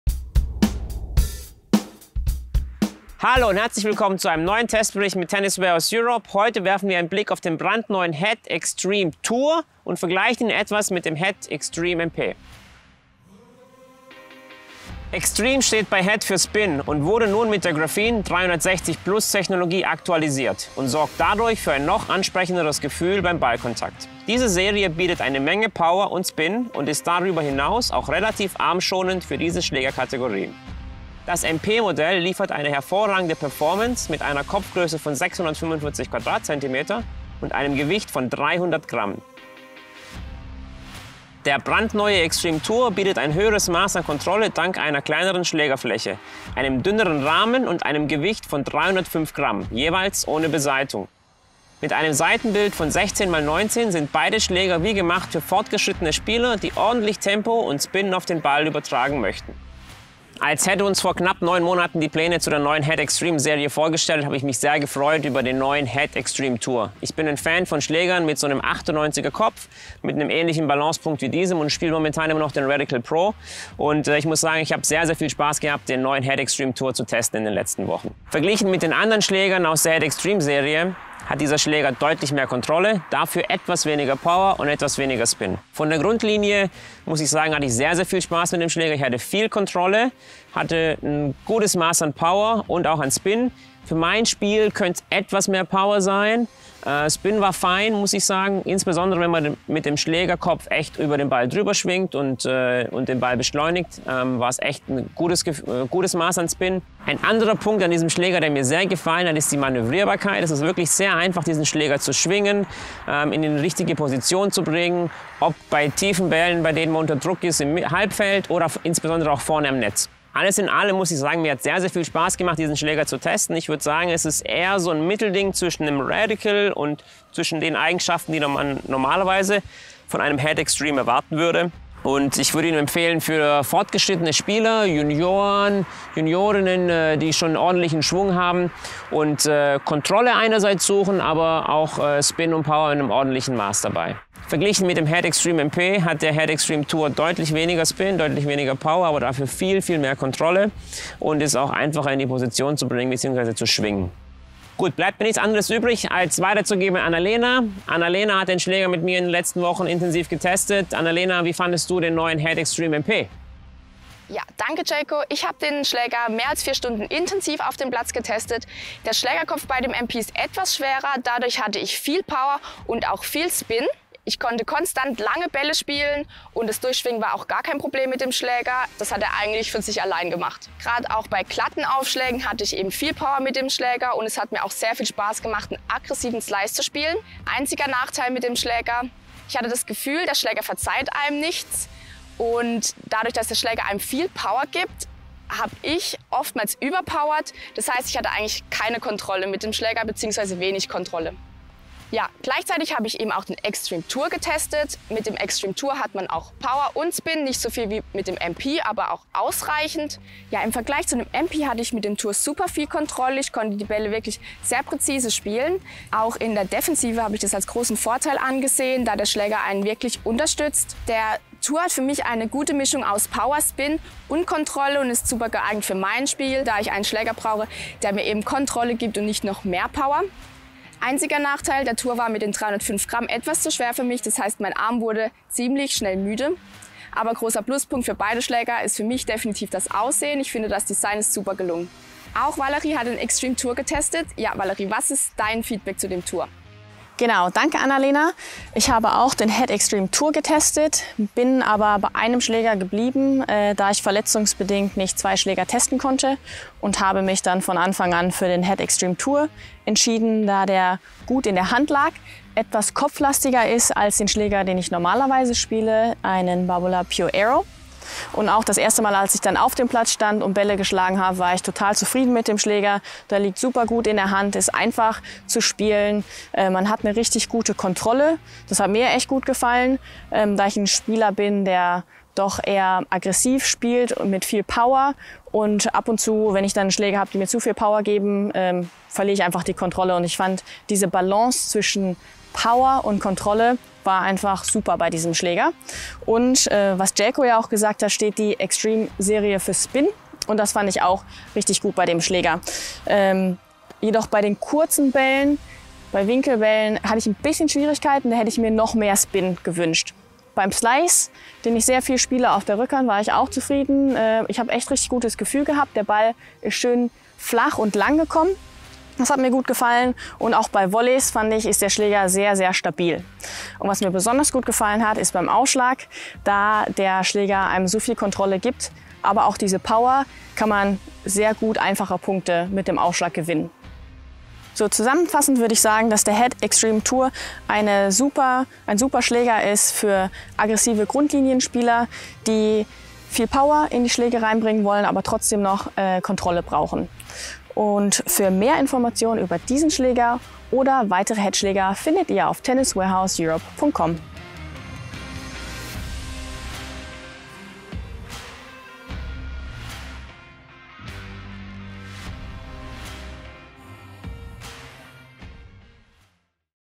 Testbericht: Head Graphene 360+ Extreme MP/ Tour Tennisschläger
Head hat seine auf Spin ausgelegte Schlägerlinie mit der Graphene 360+ -Technologie aktualisiert und sorgt dadurch für ein noch ansprechenderes Ballgefühl. Das MP-Modell liefert eine hervorragende Performance und das Tour-Modell bietet ein höheres Maß an Kontrolle.
Diese Serie bietet eine Menge Power und Spin und ist wie gemacht für fortgeschrittene Spieler, die genau das suchen.